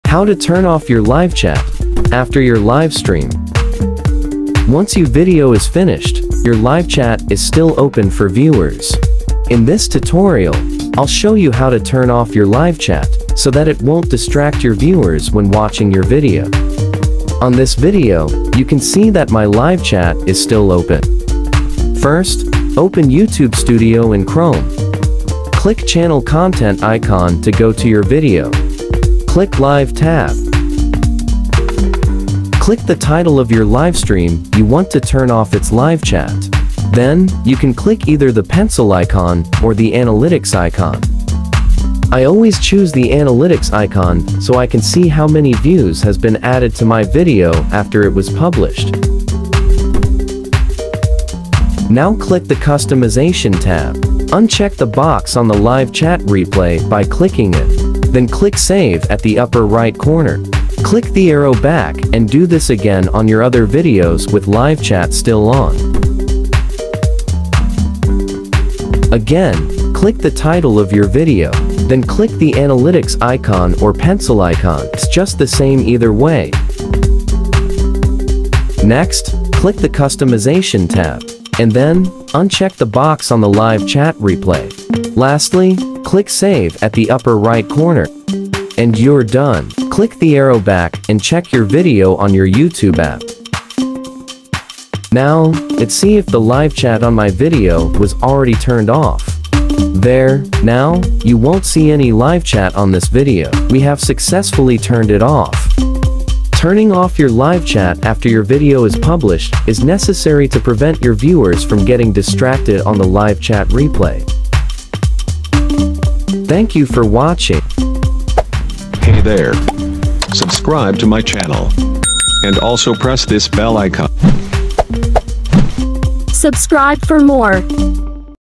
HOW TO TURN OFF YOUR LIVECHAT REPLAY | TURN OFF THE LIVECHAT REPLAY ON YOUR VIDEO | JERONE TV
In this video, I'll show you how to turn off the livechat replay on your video or your livestream. Turning off your livechat replay is just to prevent your viewers from getting distracted of the livechat replay when watching your video. Hope you get additional knowledge by watching this video.